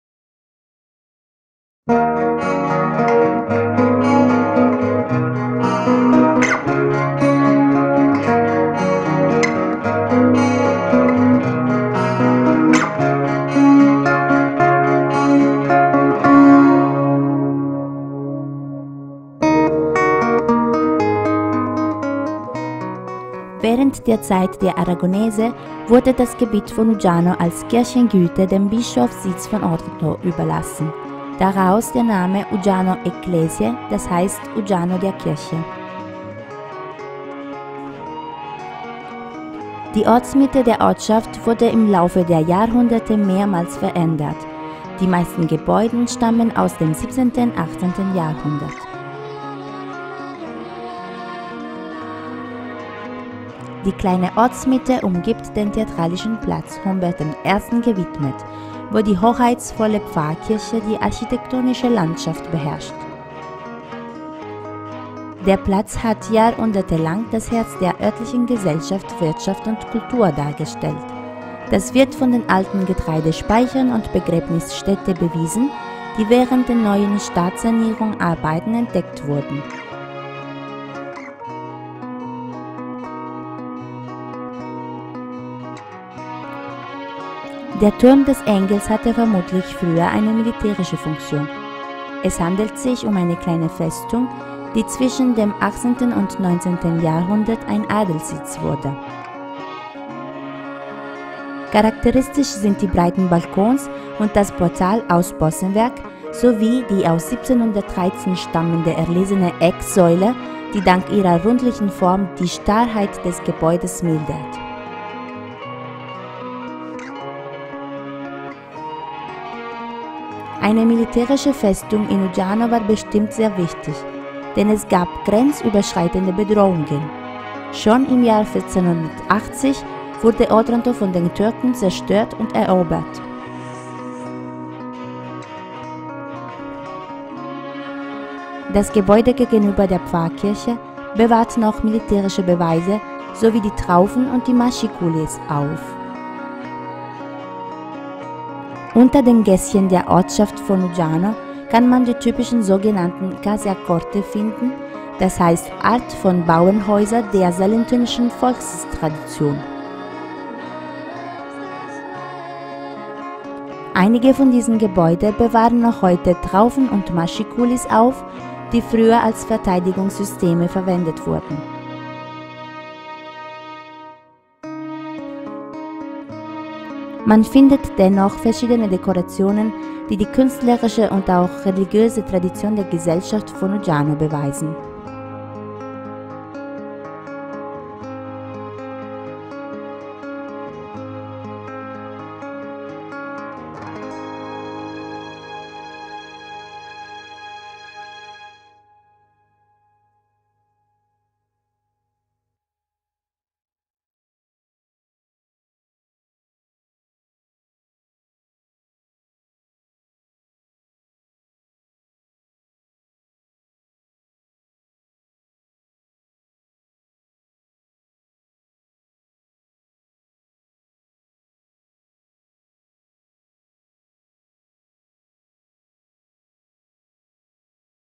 Stadt Uggiano la Chiesa - Ortsmitte
Während der Zeit der Aragonese wurde das Gebiet von Uggiano als Kirchengüte dem Bischofssitz von Otranto überlassen, daraus der Name Uggiano Ecclesiae, d.h. Uggiano der Kirche.
Die Ortsmitte der Ortschaft wurde im Laufe der Jahrhunderte mehrmals verändert. Die meisten Gebäuden stammen aus dem 17.-18. Jahrhundert.
Die kleine Ortsmitte umgibt den theatralischen Platz, Humbert I. gewidmet, wo die hoheitsvolle Pfarrkirche die architektonische Landschaft beherrscht.